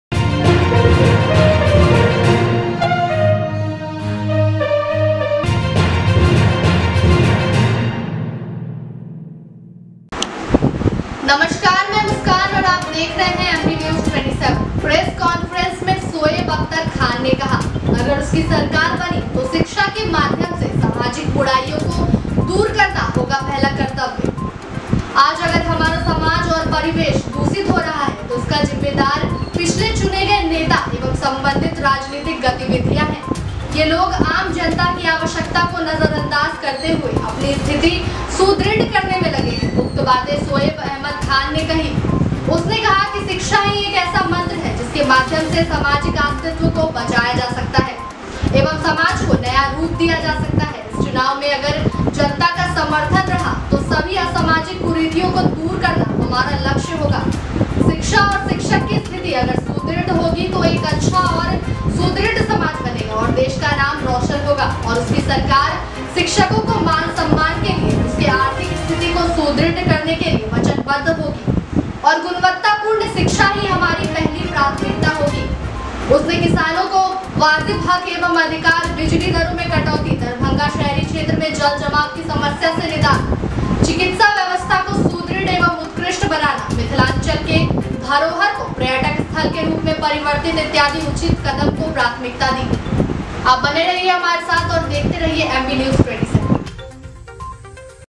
प्रेस कॉन्फ्रेंस के माध्यम से शहरी विधानसभा के आम मतदाताओं से प्रत्याशी शोएब अहमद खान ने कहा कि।
दरभंगा से,,,पत्रकार,,,राजू सिंह कि रिपोर्ट दरभंगा से ।

【प्रेस कॉन्फ्रेंस में शोएब अहमद खान ने कहा अगर उसकी सरकार बनी तो शिक्षा के माध्यम से सामाजिक बुराईयों को दूर करना होगा पहला कर्तव्य】

आज अगर हमारा समाज और परिवेश दूषित हो रहा है तो इसका जिम्मेदार पिछले चुने गए नेता एवं संबंधित राजनीतिक गतिविधियां हैं । ये लोग आम जनता की आवश्यकता को नजरअंदाज करते हुए अपनी स्थिति सुदृढ़ करने में लगे हैं। उक्त बातें शोएब अहमद खान ने कही।उसने कहा- शिक्षा ही एक ऐसा मंत्र है जिसके माध्यम से सामाजिक अस्तित्व को बचाया जा सकता है एवं समाज को  नया रूप दिया जा सकता है । इस चुनाव में अगर जनता का समर्थन रहा तो सभी असामाजिक कुरीतियों को दूर करना हमारा लक्ष्य होगा। शिक्षा और शिक्षक की स्थिति अगर सुदृढ़ होगी तो एक अच्छा और सुदृढ़ समाज बनेगा और देश का नाम रोशन होगा।और उनकी सरकार शिक्षकों को मान सम्मान के साथ उनके आर्थिक स्थिति को सुदृढ़ करने के लिए वचनबद्ध होगी और गुणवत्तापूर्ण शिक्षा ही हमारा पहली प्राथमिकता होगी।उसने किसानों को वाजिब हक एवं अधिकार,  बिजली दरों में कटौती, दरभंगा शहरी  क्षेत्र में जल जमाव की समस्या से निदान, चिकित्सा व्यवस्था को सुदृढ़ एवं उत्कृष्ट बनाना,  मिथिलांचल के धरोहर को पर्यटक स्थल के रूप में परिवर्तन इत्यादि उचित कदम को प्राथमिकता दी।